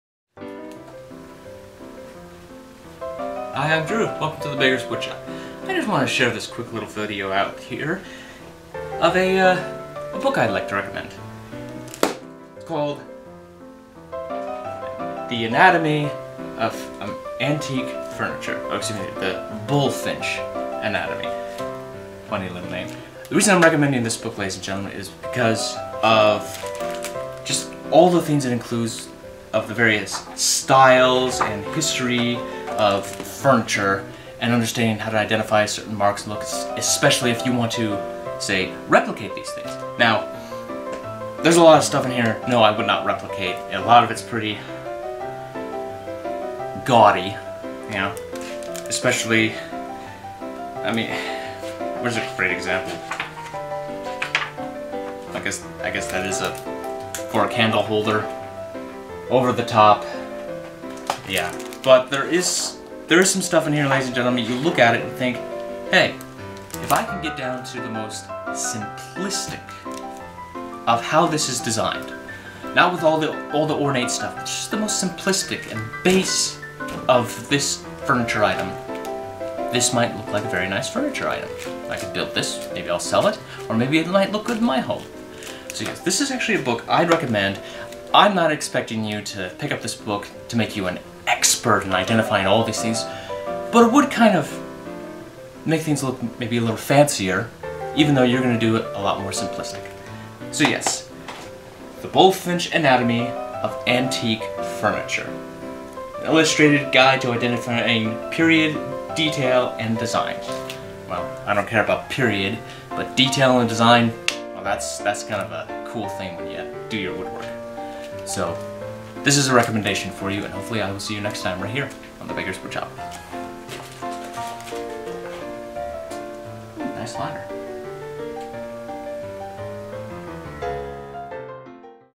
The Beggar Recommends a Book
I recommend to you The Bulfinch Anatomy of Antique Furniture by Tim Forrest.